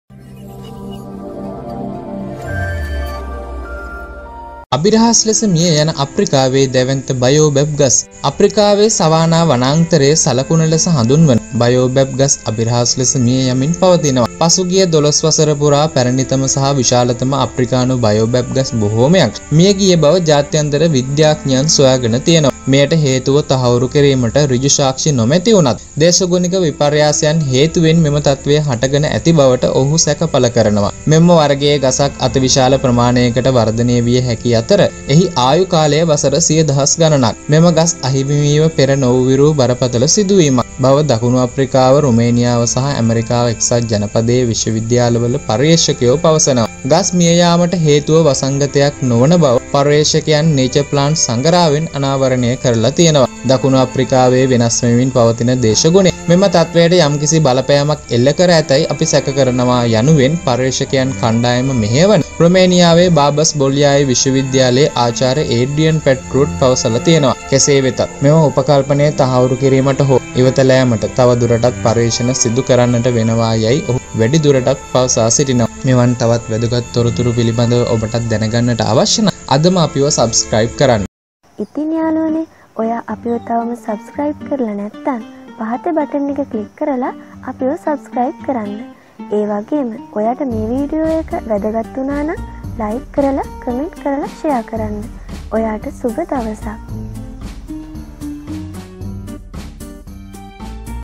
මේ එක ගසක් කෝටි ප්‍රකෝටි ගානක් වටිනව ඔබත් ගසක් හිටව ගන්න - Ancient Baobab Trees in Africa
Ancient Baobab Trees in Africa

අප්‍රිකාවේ සවානා වනාන්තරයේ සලකුණ ලෙස හඳුන්වන 'බයෝබැබ්' (Baobab) ගස් අබිරහස් ලෙස මිය යමින් පවතී.

පසුගිය දොළොස් වසර පුරා පැරණිතම සහ විශාලතම අප්‍රිකානු බයෝබැබ් ගස් බොහෝමයක් මිය ගිය බව ජාත්‍යන්තර විද්‍යාඥයන් සොයාගෙන තිබේ.

මෙයට හේතුව තහවුරු කිරීමට ඍජු සාක්ෂි නොමැති වුවත්, දේශගුණික විපර්යාසයන් හේතුවෙන් මෙම තත්වය හටගෙන ඇති බවට ඔවුහු සැක පහළ කරති.

මෙම වර්ගයේ ගසක් අති විශාල ප්‍රමාණයකට වර්ධනය විය හැකි අතර එහි ආයු කාලය වසර සිය දහස් ගණනකි.